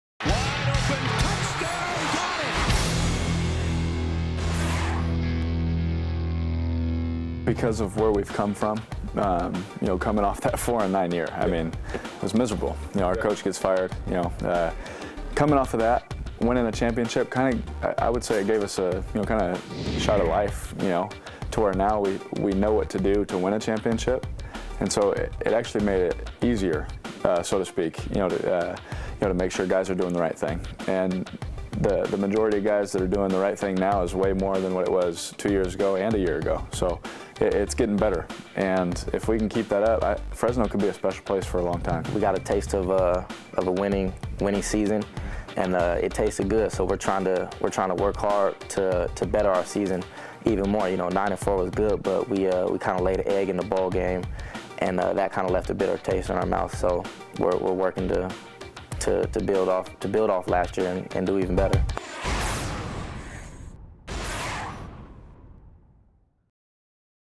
MWFB Media Day: Derek Carr and Derron Smith, Fresno State
Jesse Kurtz of the Mountain West Network chats with Fresno State senior quarterback Derek Carr and junior free safety Derron Smith at the 2013 MW Football Media Days, held at The Cosmopolitan of Las Vegas.